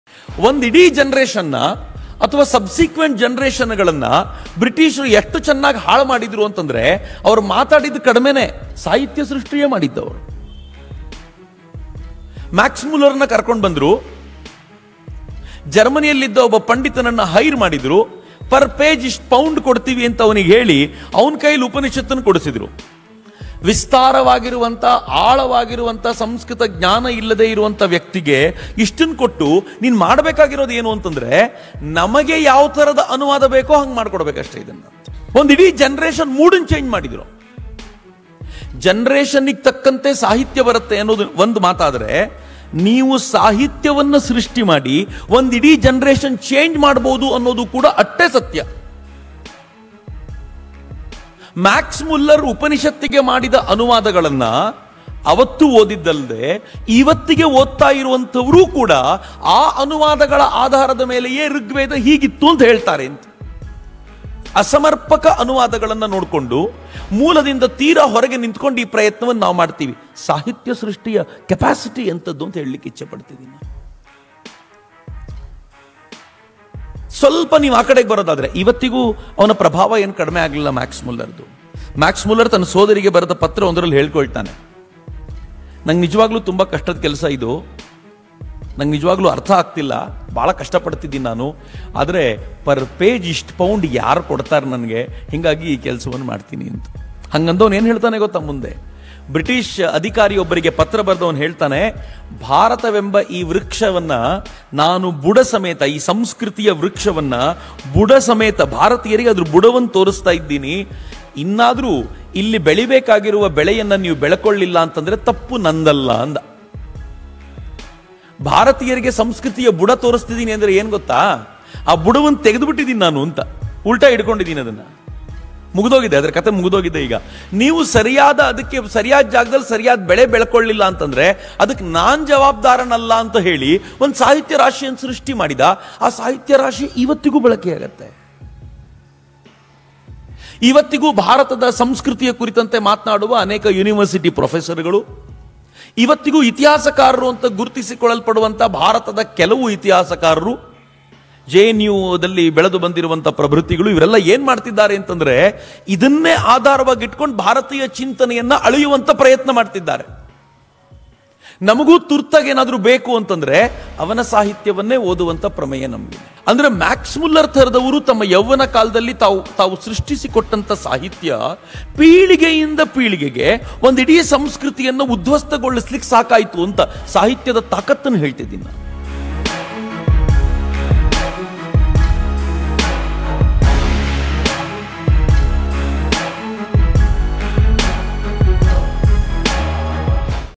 ಸಾಹಿತ್ಯದಿಂದ ಭಾರತೀಯ ಸಂಸ್ಕೃತಿಯನ್ನು ಬುಡಮೇಲು ಮಾಡಿದ ಮ್ಯಾಕ್ಸ್ ಮುಲ್ಲರ್ | ಚಕ್ರವರ್ತಿ ಸೂಲಿಬೆಲೆ
ಸಾಹಿತ್ಯದಿಂದ ಭಾರತೀಯ ಸಂಸ್ಕೃತಿಯನ್ನು
ಬುಡಮೇಲು ಮಾಡಿದ ಮ್ಯಾಕ್ಸ್ ಮುಲ್ಲರ್ |
ಚಕ್ರವರ್ತಿ ಸೂಲಿಬೆಲೆ